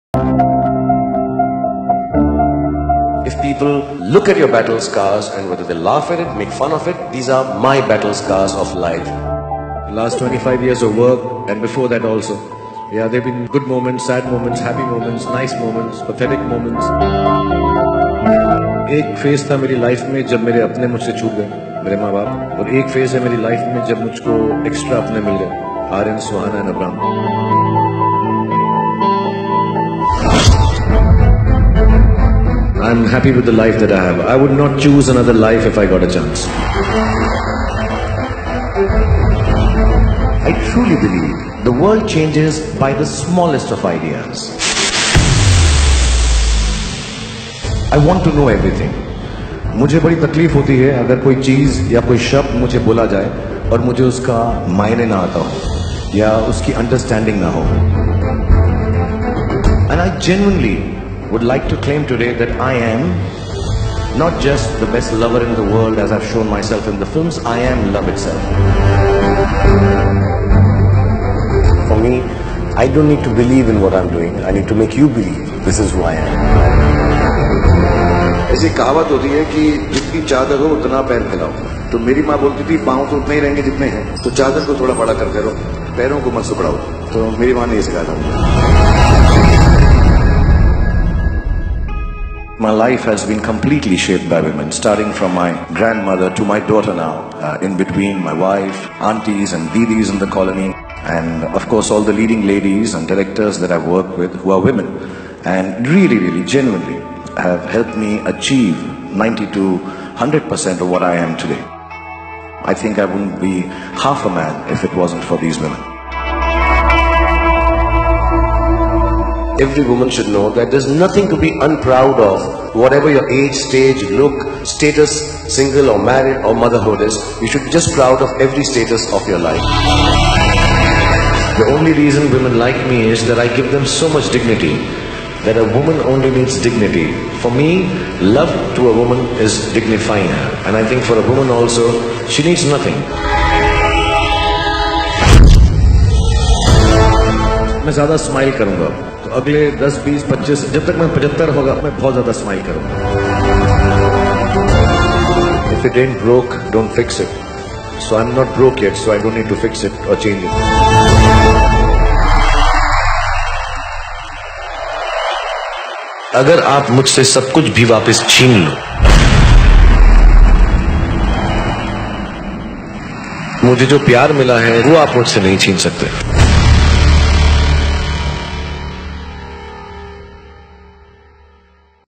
"My Life completely shaped by women"
My life has been shaped by women: Shah Rukh Khan

"My life has been shaped by women – my grandmother, mother and now my daughter…," said Bollywood superstar Shah Rukh Khan. "I would not be even half a man without them."

He was speaking at a function organised to launch the book She Walks, She Leads authored by Gunjan Jain. The book, published by Penguin Random House India, chronicles the extra-ordinary journeys of 24 women leaders in India who have fought their way to attain success in their chosen fields.